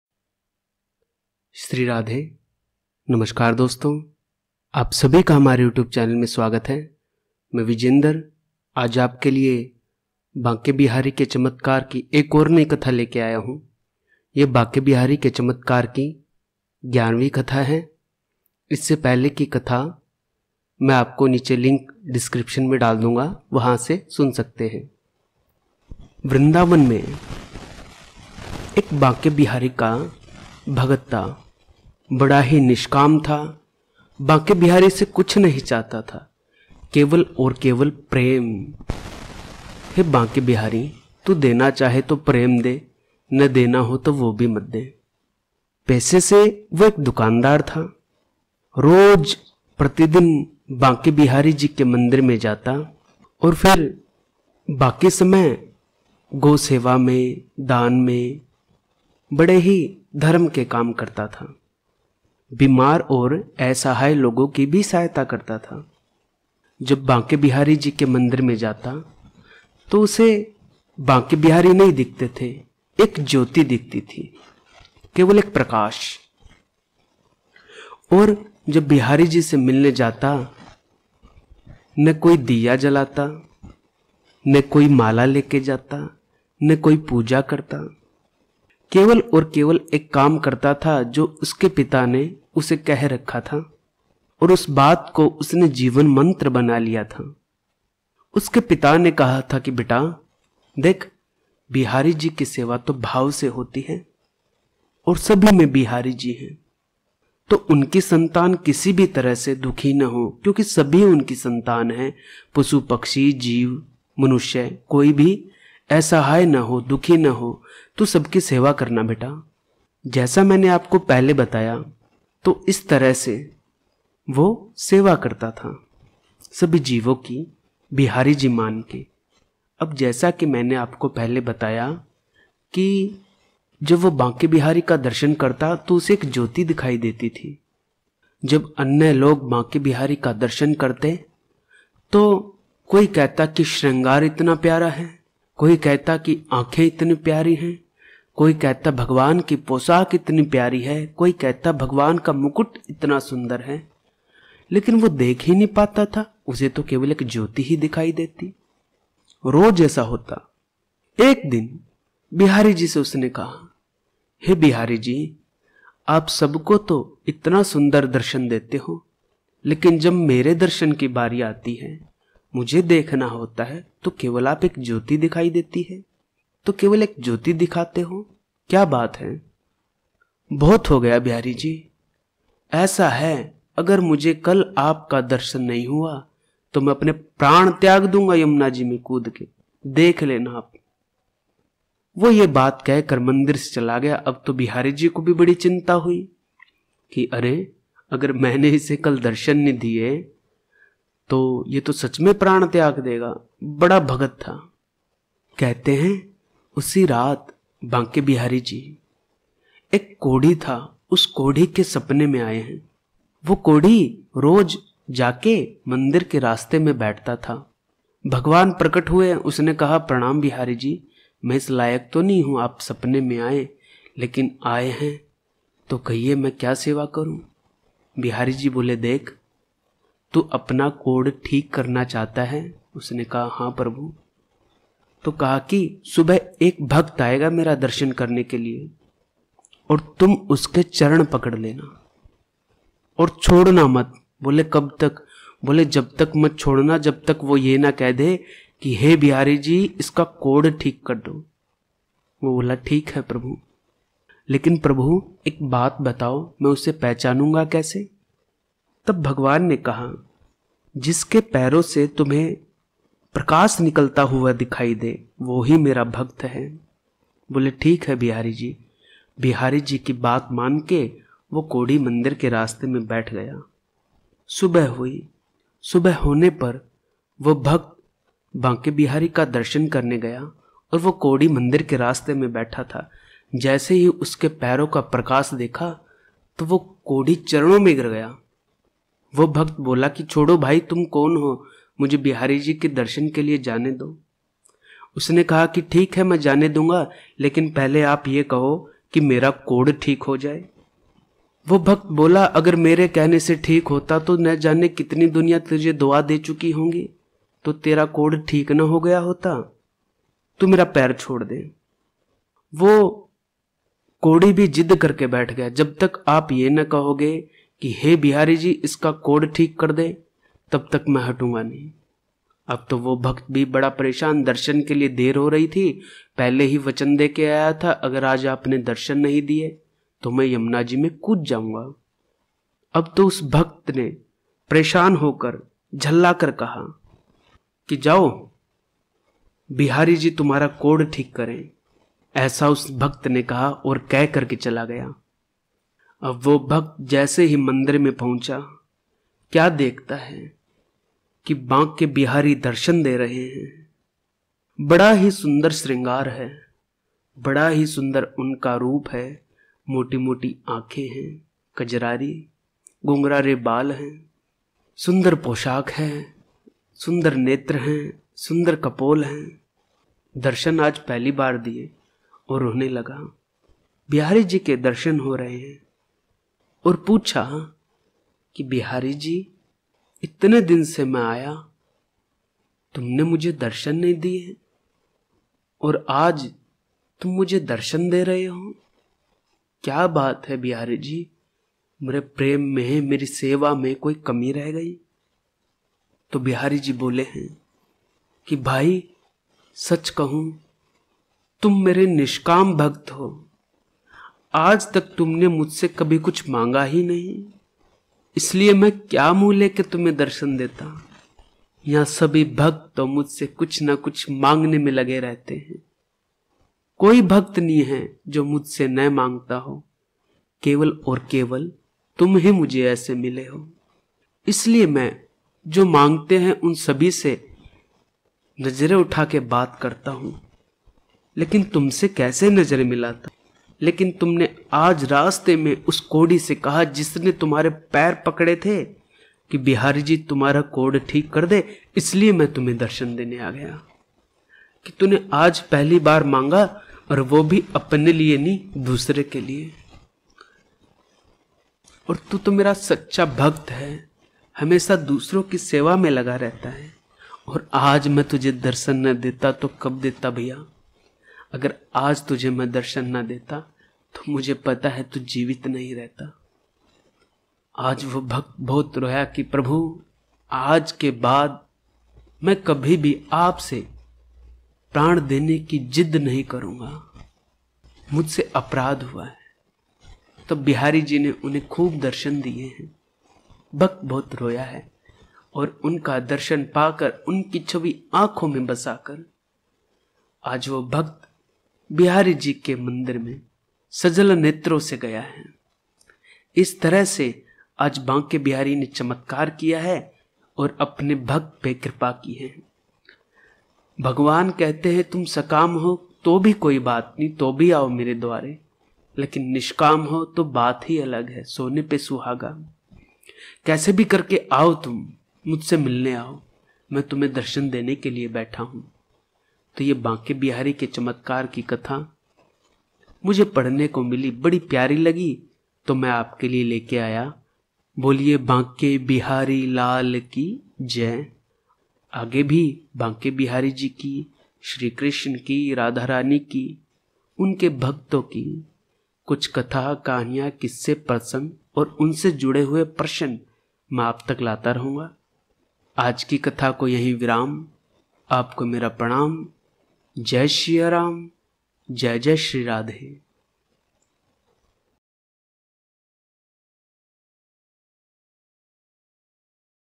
Banke Bihari Chamatkar Story/Kahani/Katha 11 || बांके बिहारी चमत्कार कथा : 11 || Audio Story
Yahan Par Shri Banke Bihari ke Chamatkar ki 11vi Katha di ja rahi hai. Ek Banke Bihari ji ka Bhakt hota hai. Vo bhakt Banke Bihari ke mandir me roj Darshan karne jaata hai. Lekin use Bihari ji ke darshan nahi hote hai. Keval ek Jyoti Dikhai deti hai. Ab to is Bhakt ko Dukh hua ki mujhe Bihari ji Darshan kyo nhi dete hai. Tab Banke Bihari ne aisi kaunsi tarkib nikali ki us bhakt ko darshan diye. Boliye Banke Bihari Lal ki Jai.